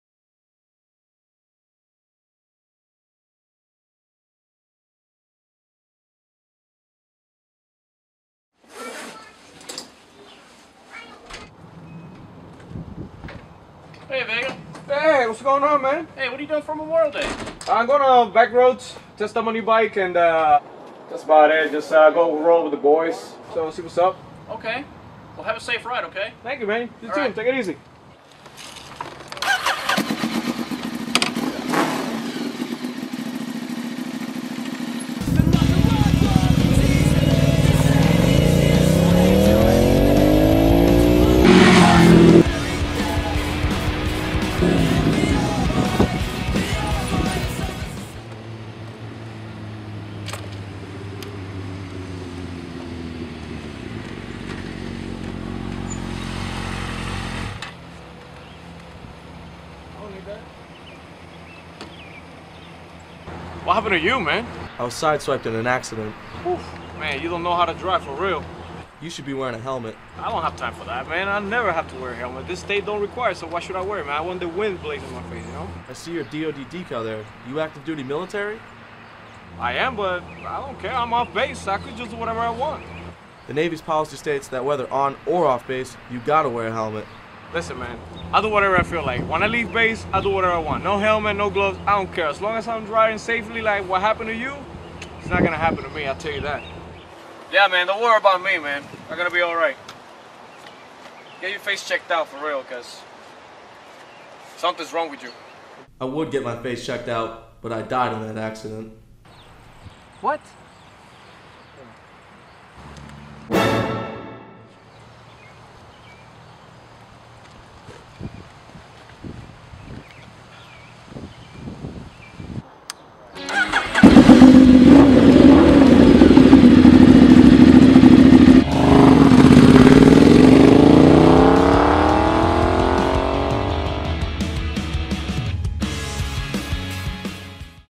CSADD: Use Your Head, Wear A Helmet
Coalition of Sailors Against Destructive Decisions (CSADD) Motorcycle Safety Video: Use Your Head, Wear A Helmet
U.S. Navy video from Commander, Submarine Group Two